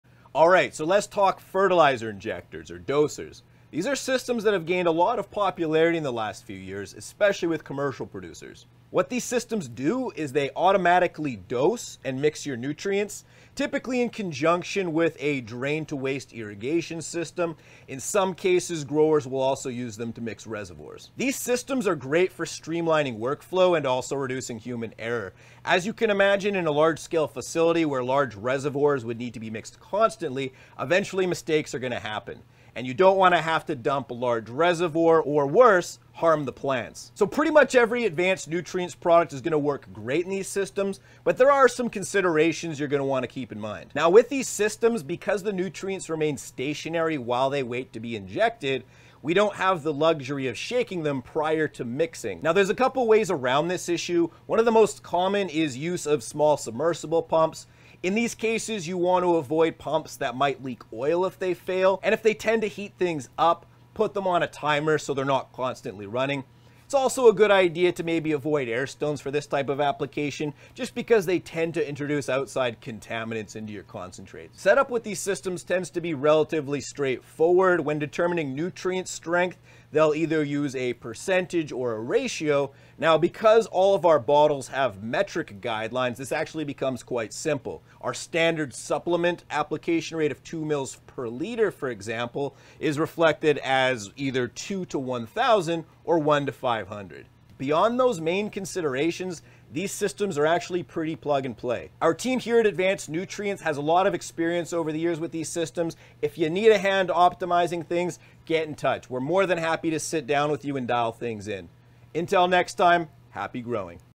Ask An Expert - Fertilizer Injectors (Dosers)
Let’s Talk Fertilizer Injectors

As we optimize and scale our grows, it becomes increasingly important to reduce human error. Follow along as our cultivation expert Rory shows how you can cut back on costly mistakes using dosers for your production.

Got a grow question you want answered in an upcoming episode? Share it with us in the comments below!